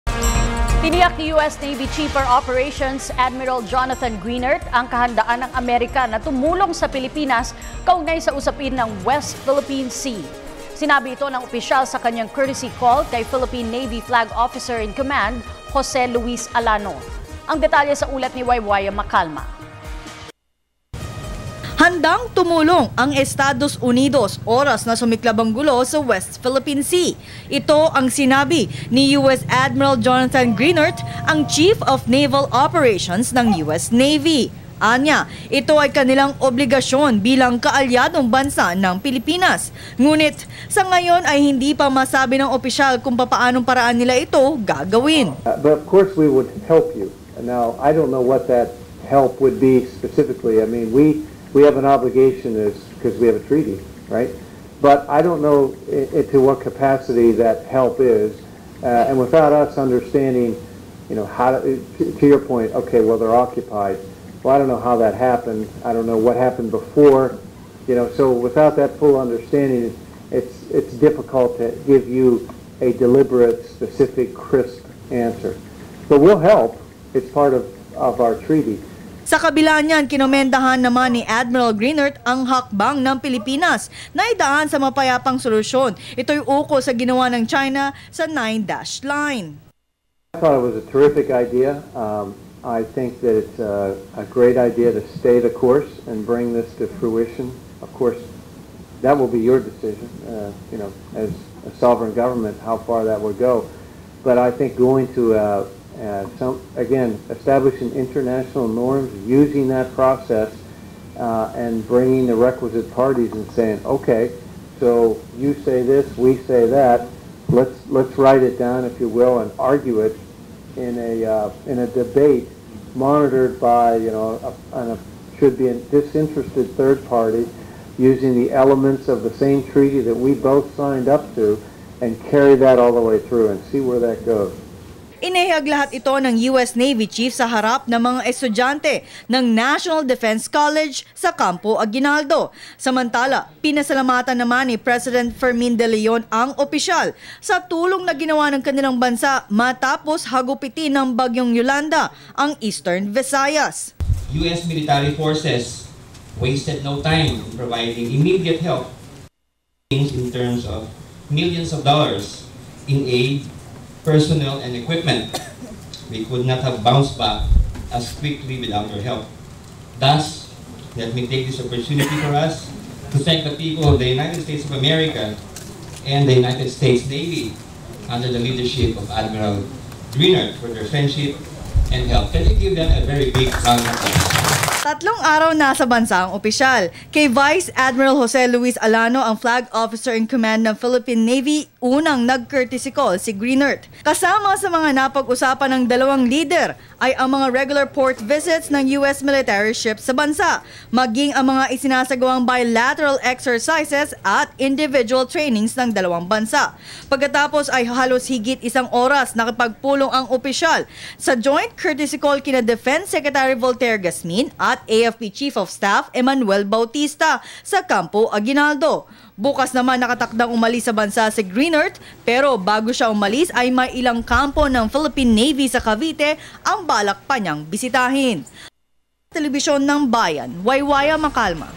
US Navy Admiral Greenert: Amerika, handang tumulong sa Pilipinas sa isyu ng West PHL Sea
News@6: US Navy Admiral Jonathan Greenert: Amerika, handang tumulong sa Pilipinas sa isyu ng West PHL Sea (Ulat ni Waywaya Macalma) - [February 13, 2014]